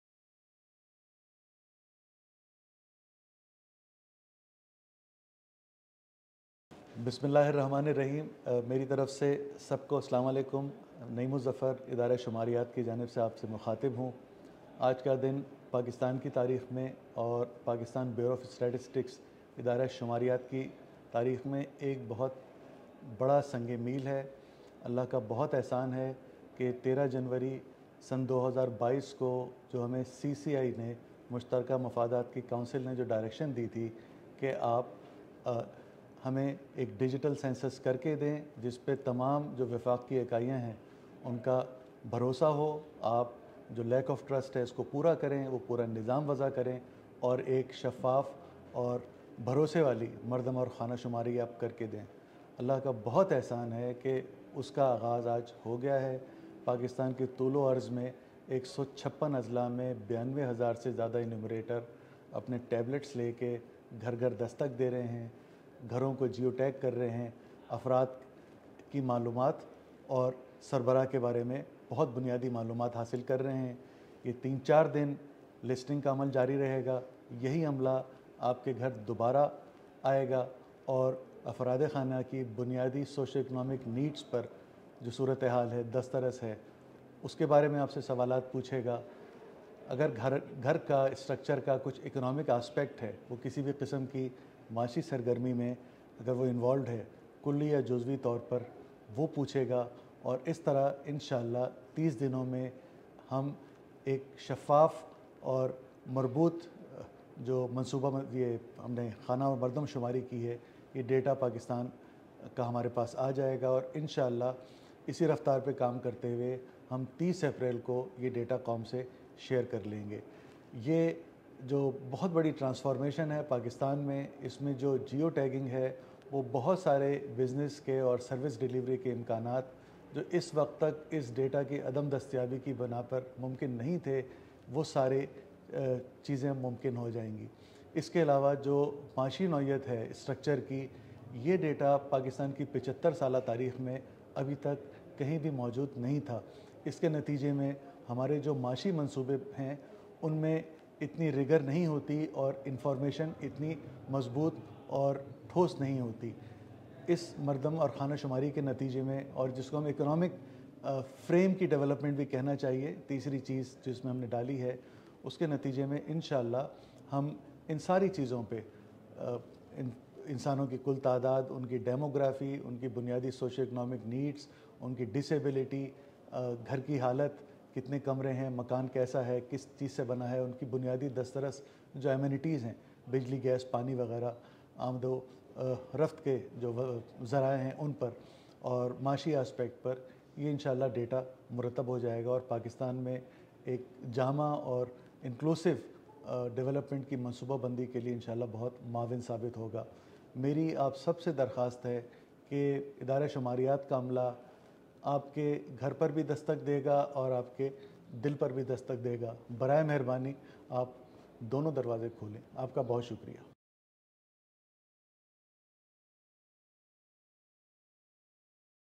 Chief Census Commissioner, Dr Naeem Uz Zafar
Chief Census Commissioner, Dr Naeem Uz Zafar, encourages all of Pakistan to fully participate in the 7th Population and Housing Census and get enumerated!